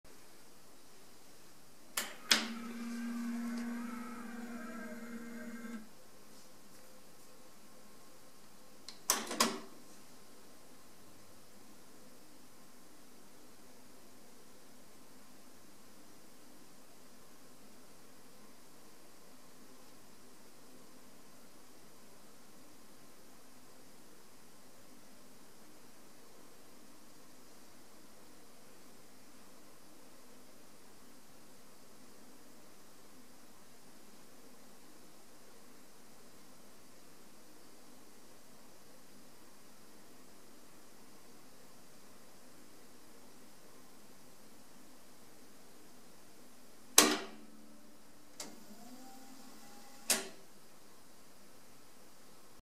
Luxman KX-102 aka "Suck-Face" Calibration
Extremely rare three-head beauty with suck-face (sliding panel).